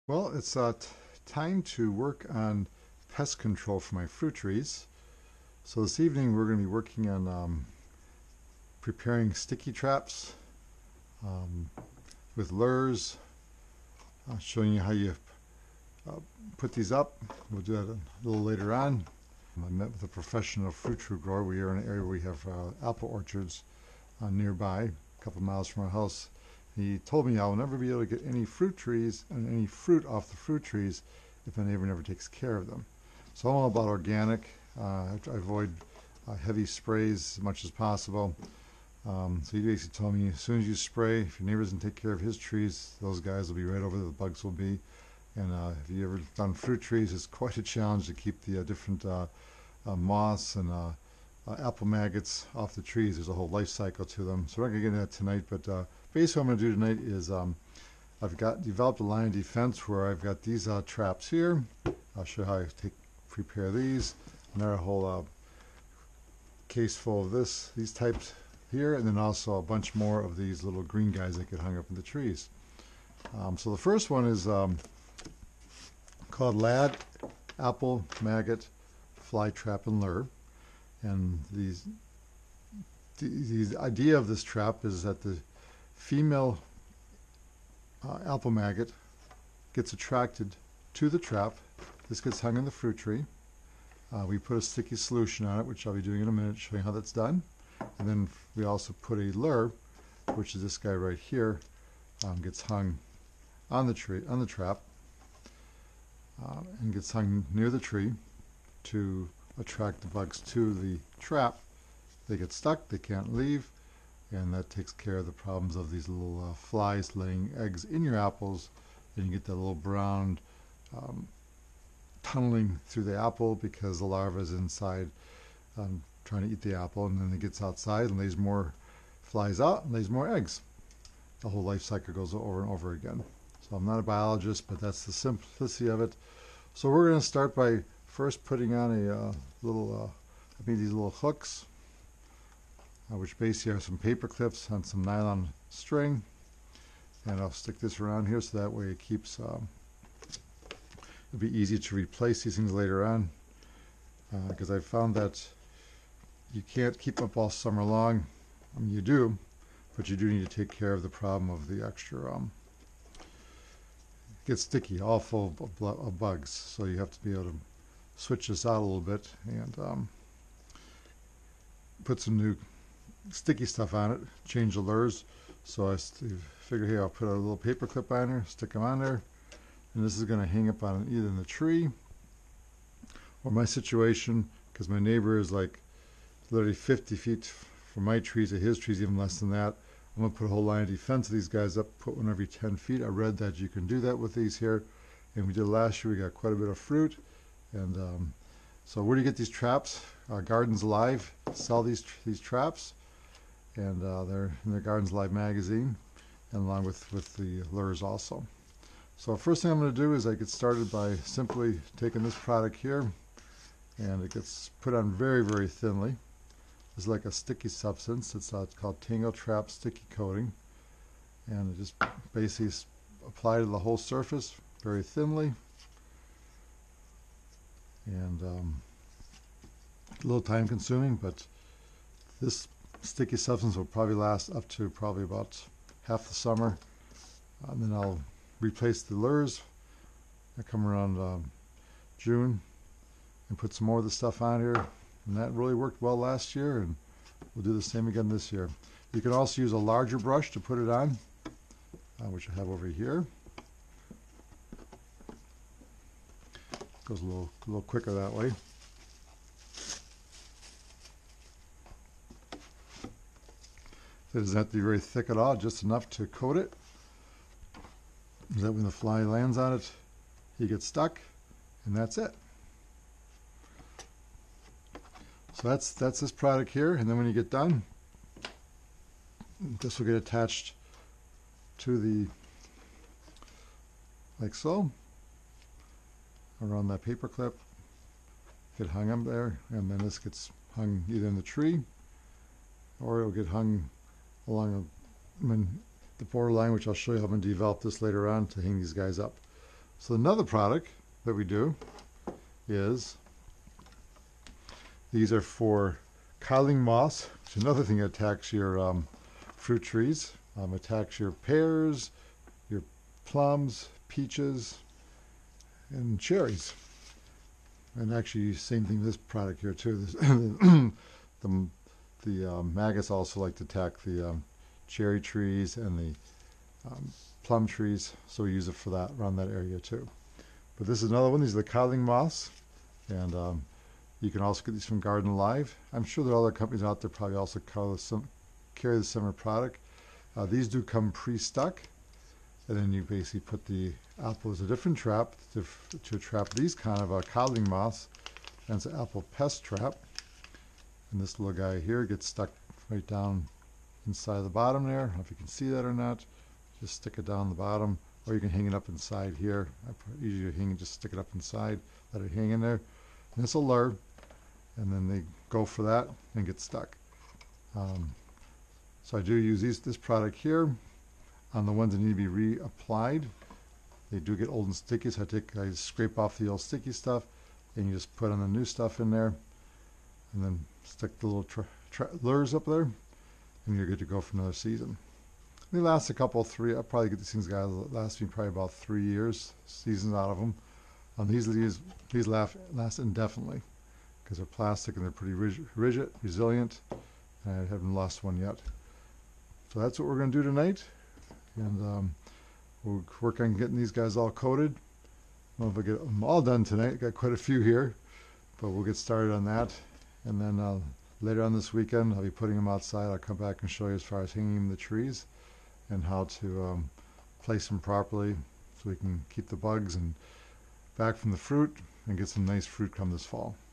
How To Protect Fruit Trees from Pesky Worm/Moths and Bug Damage - Simply Garden
Protect fruit trees from worms, moths and bugs by coating apple maggot fly traps along with apple pest traps. Then place lures on the traps. Using tangle trap sticky coating on traps will ensure protection for your apple, pear, peach, plum and cherry trees so to produce healthy fruit crops. Best results to prepare traps and set out in early spring. Replace lures every 4-8 weeks. Reuse traps year after year.